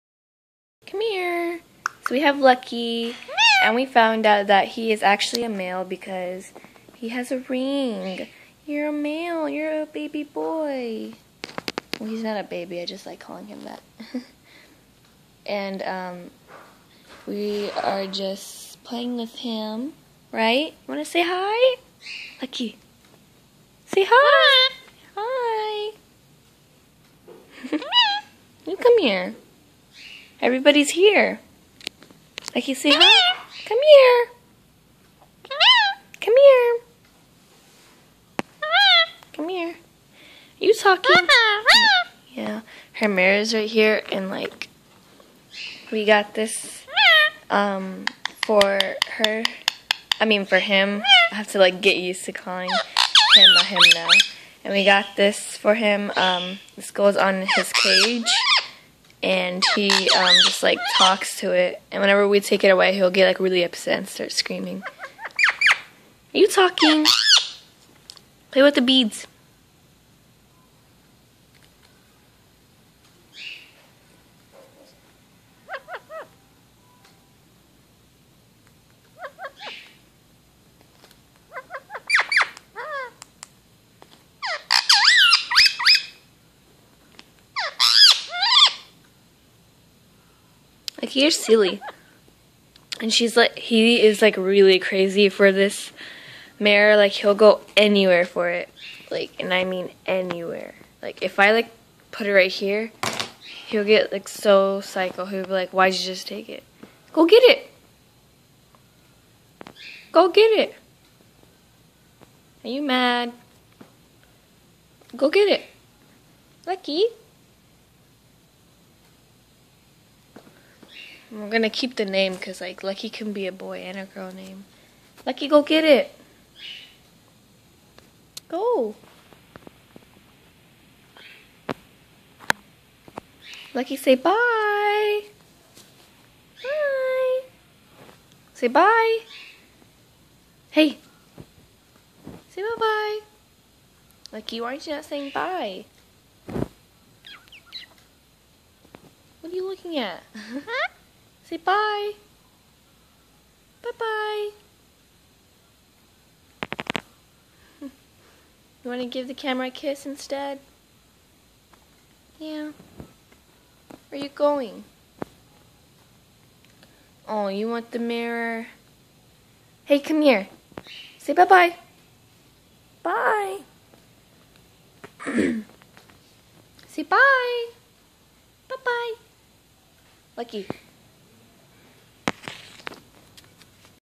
Lucky talking to himself!
So today lucky was talking to himself and we also found out that lucky is a male and not a female lol!